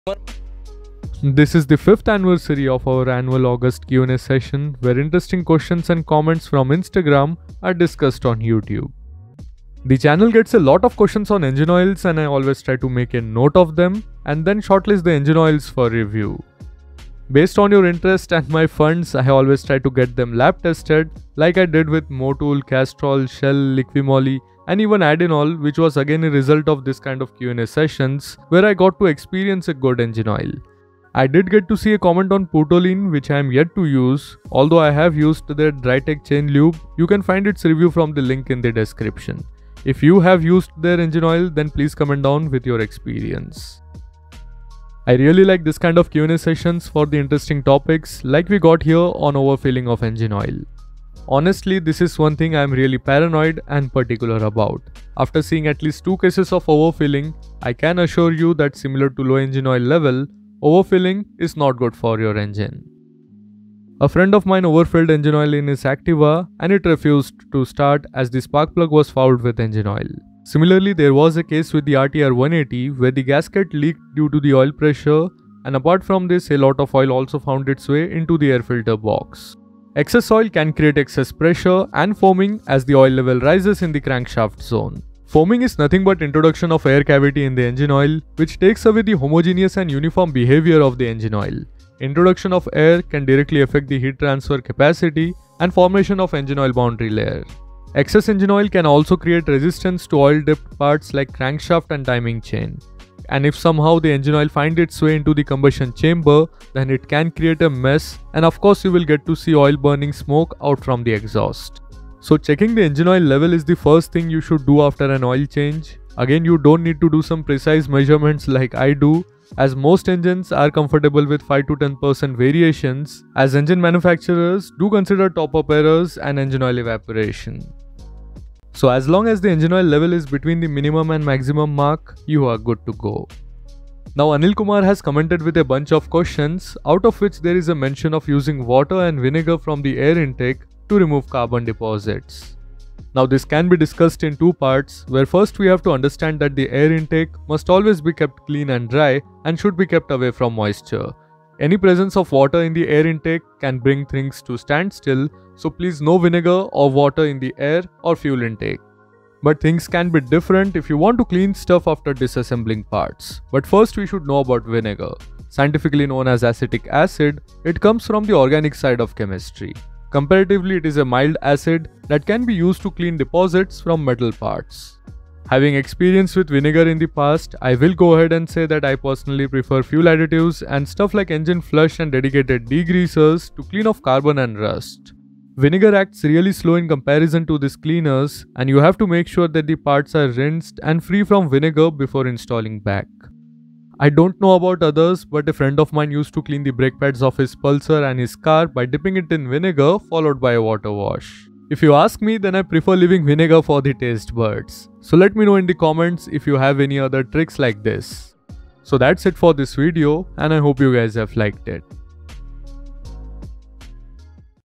WHY NEVER OVERFILL ENGINE OIL & USE VINEGAR TO CLEAN ENGINE, AIR & FUEL INJECTION
What happened when my friend overfilled his engine with engine oil! Added extra engine oil? Engine oil level above the maximum level? Too much engine oil in engine? What happens when you overfill engine oil in your car or motorcycle? Can we use vinegar to clean the engine? Vinegar engine flush

This is our August question and answer session where we discuss on Instagram comments.

Tool kit 1

Tool kit 2